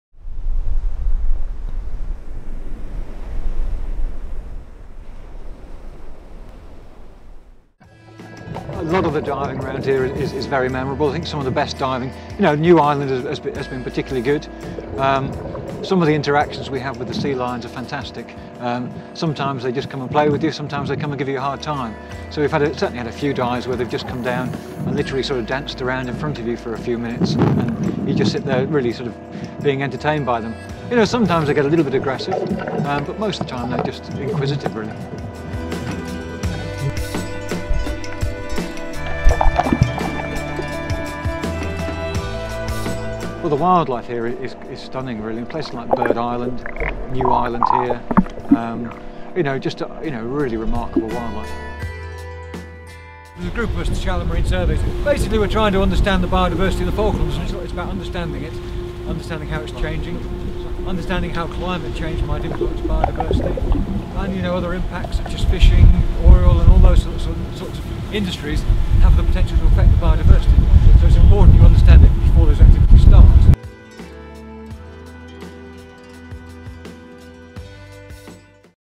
The Falklands Experience - Martin
As well as his role as CEO of South Georgia Government, Martin Collins is part of the Shallow Marine Survey Group, investigating the biodiversity of Falklands waters. Step beneath the waves and discover the underwater world of the Falkland Islands.
Film by Oly Dempster, shot by Duncan Lees & Oly Dempster.
Additional footage by Martin Collins & Jerry Pierce.
Commissioned on behalf of the Falkland Islands Government.